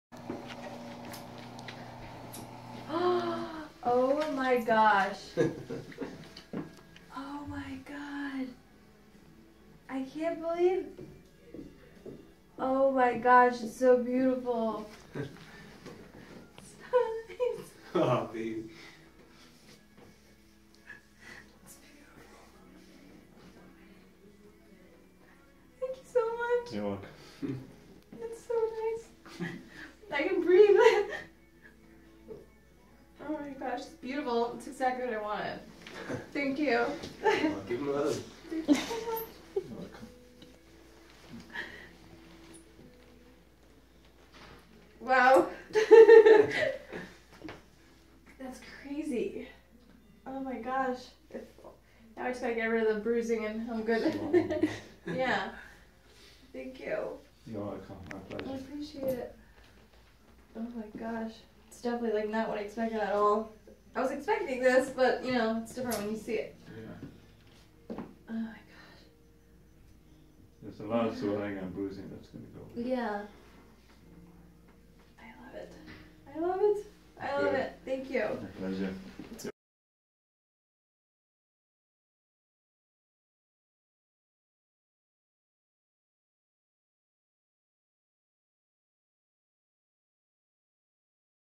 M.T's journey
In this video Mehir sees her new nose for the first time.